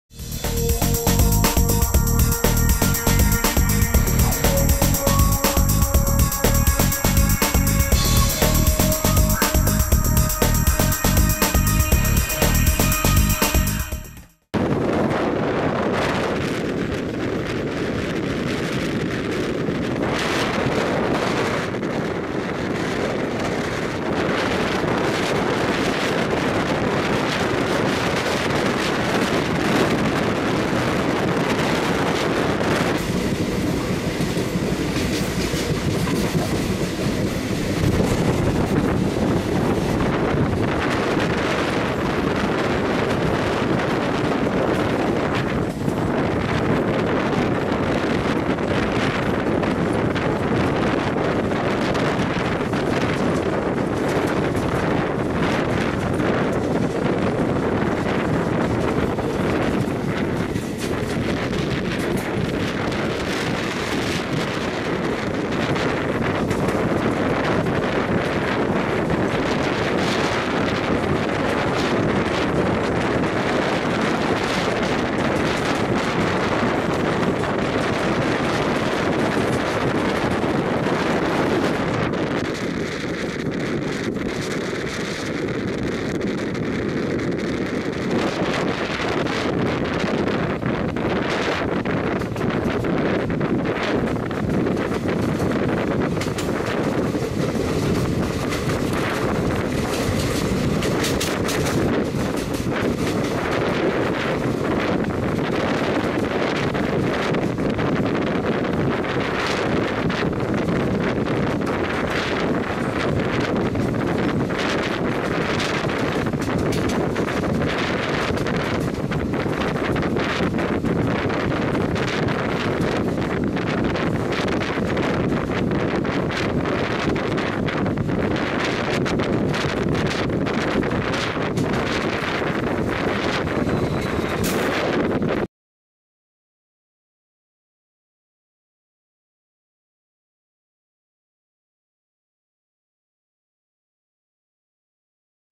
Poorna Cruises Beyond Kulem
After getting rid of the brakers at Kulem, Ernakulam bound Poorna Express coasts amidst greenery toward Kudchade